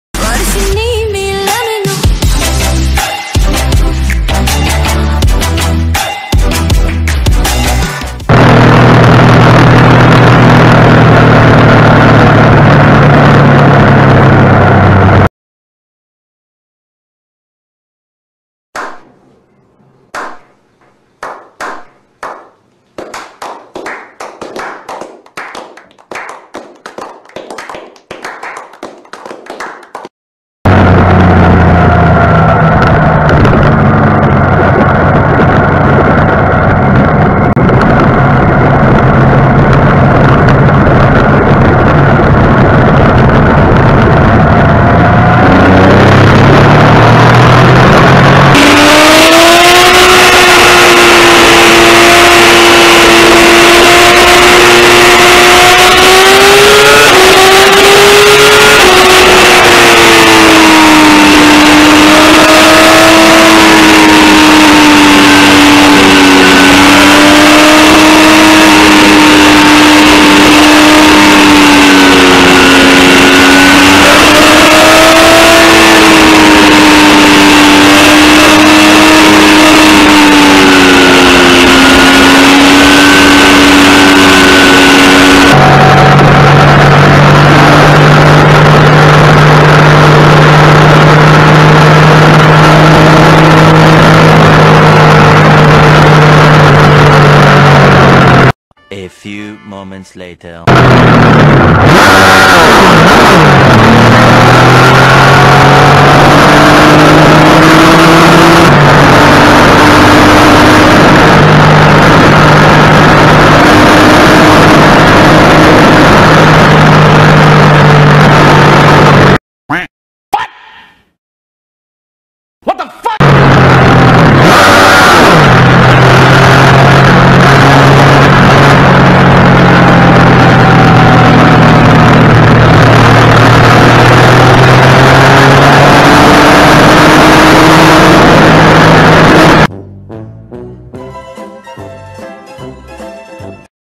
Closed Call Yamaha MT09 vs myvi & BMW
Almost got hit...wtf